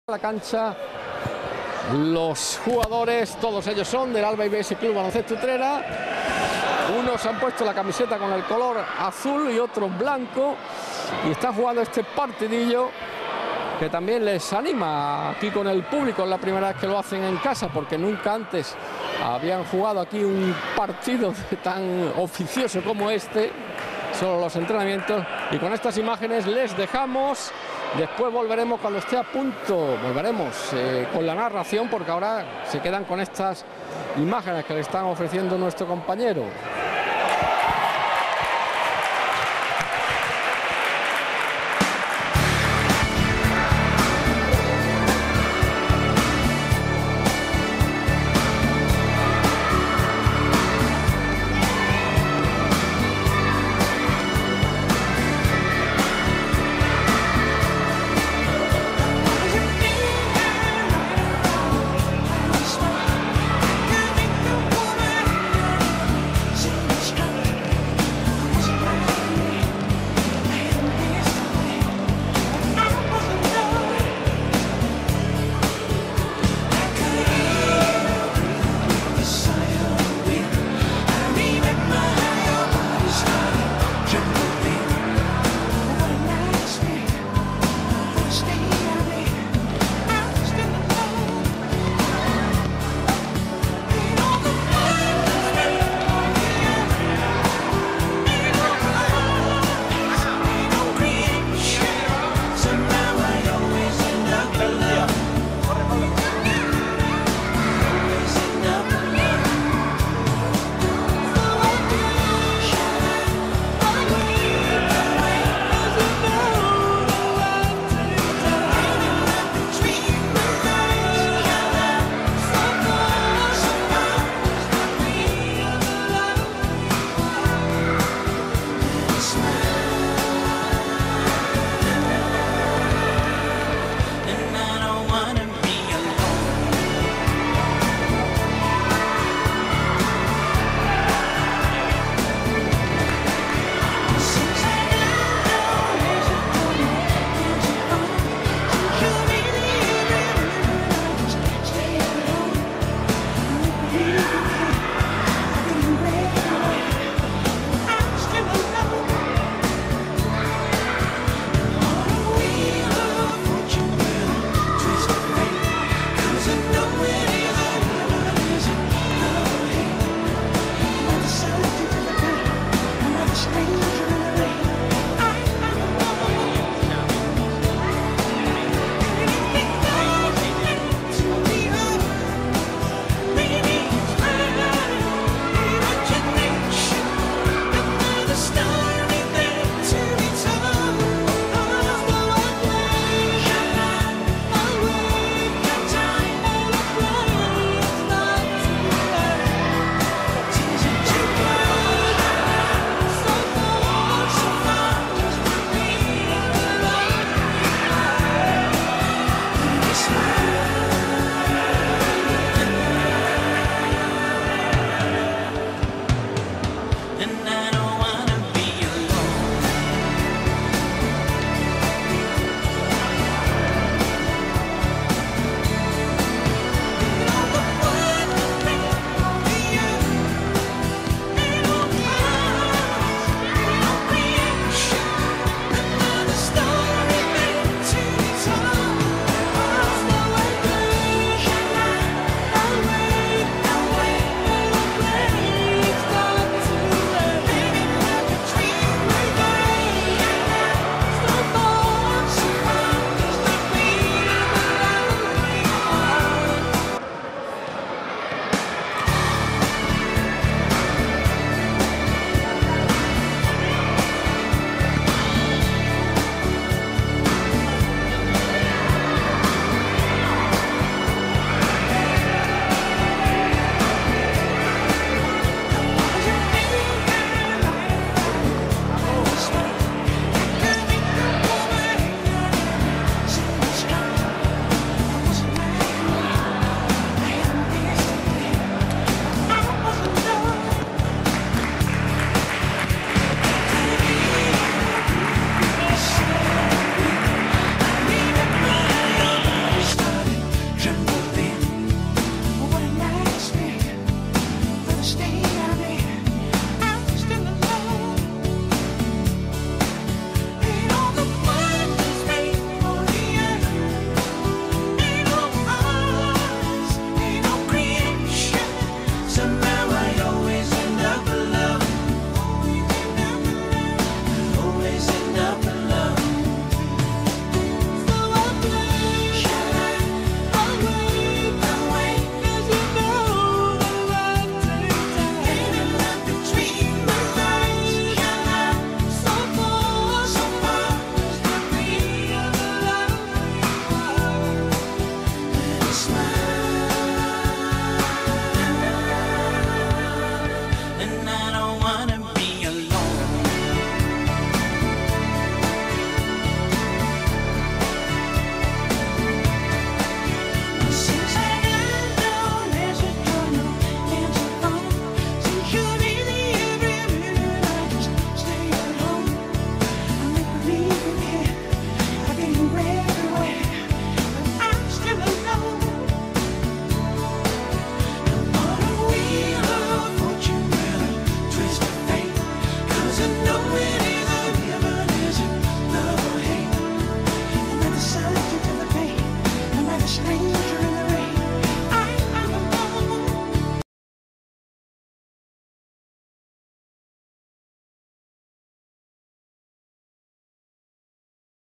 Club Baloncesto Utrera equipo Baby Basket 2018
Video con imágenes del partidillo disputado por los intengrantes de la plantilla del equipo de Baby-basket del Club Baloncesto utrera en mayo de 2018. --